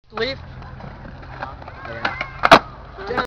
Clean 360 Flip
Antonios clean tre flip at ST. Robert skate park